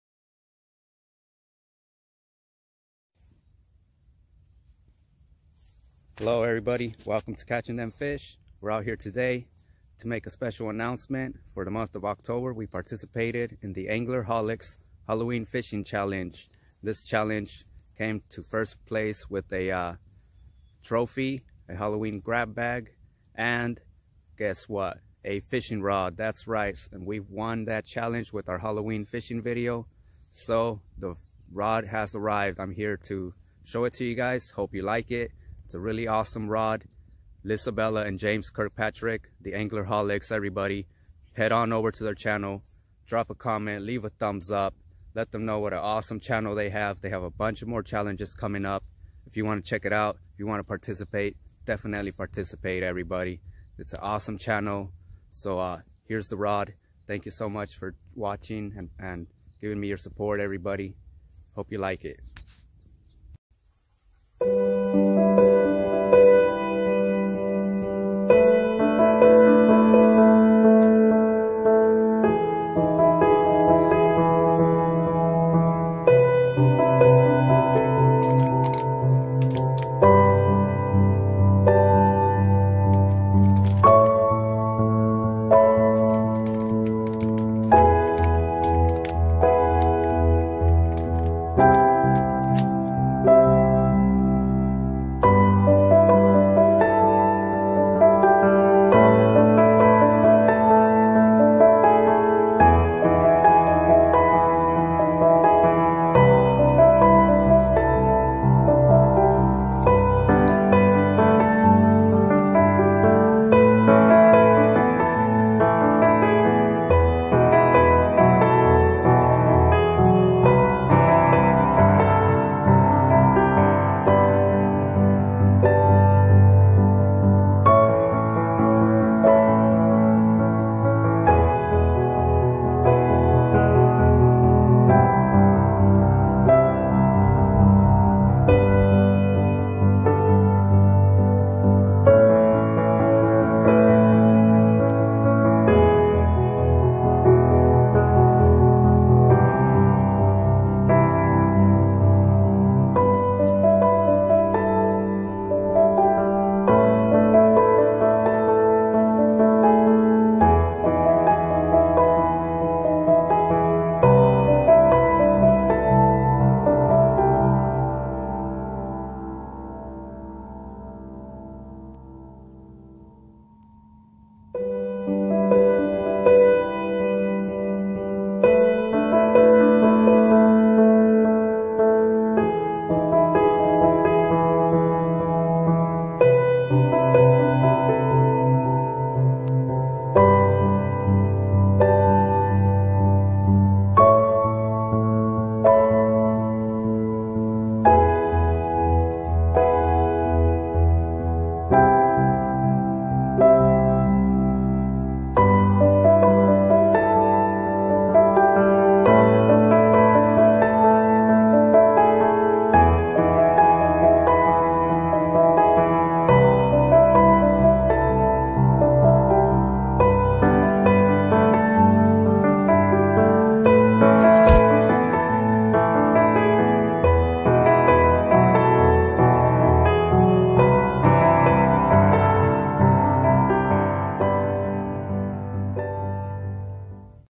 Mail call #2
On this Mail call i will be showing you The Rippin Lips Super Cat Rod. It is a Prize i won by participating in the Anglerholics Halloween Costume Fishing Challenge.

Make sure you check out thier channel!

Anglerholics here on youtube

Lizabella and James Kirkpatrick Thankyou so much I love my new Favorite rod!

Made with - YouCut video editor